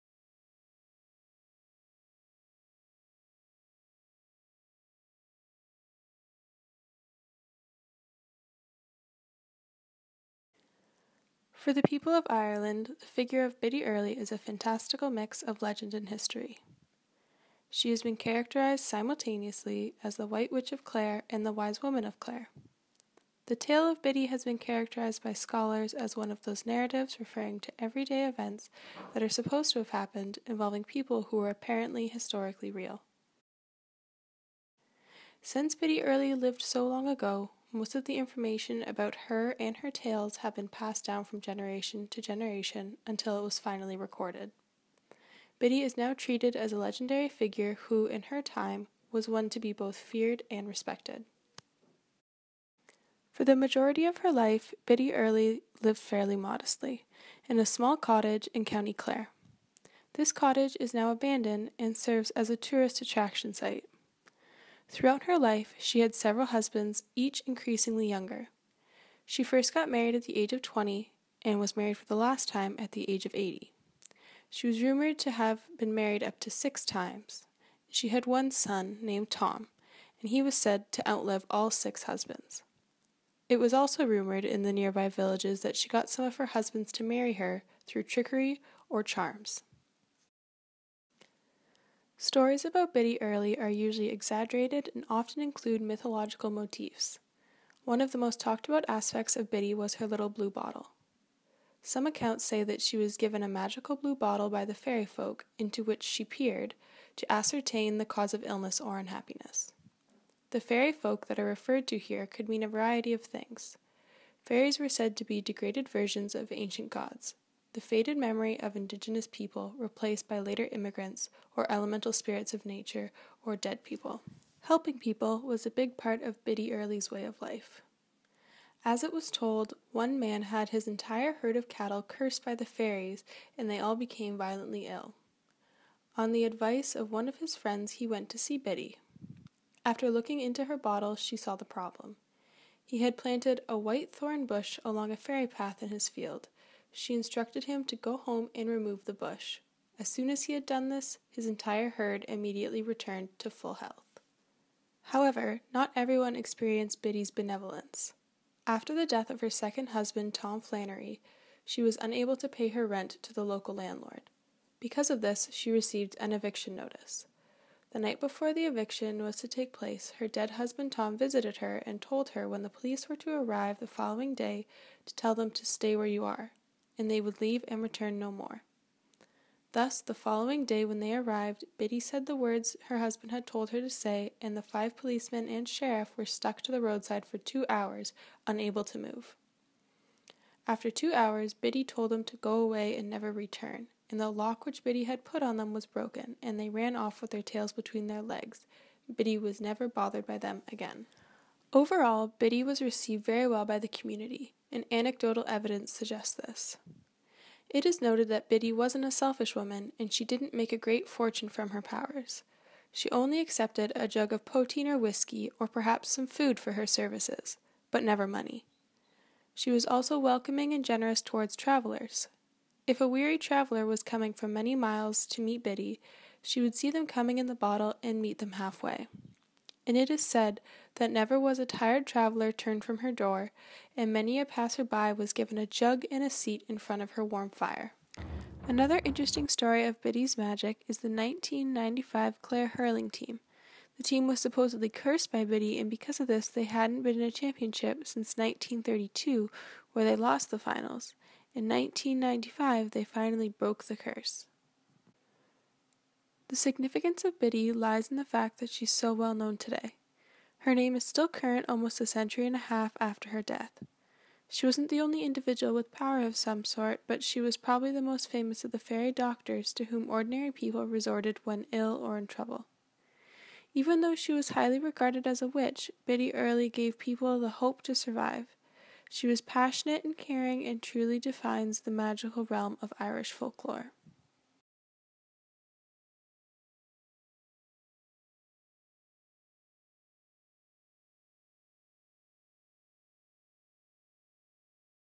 Biddy Early Folklore
A project on Biddy Early, an Irish Wisewoman living in the 19th century.

By: Hannah Cohen, Melissa Walters, and Madeline Barnes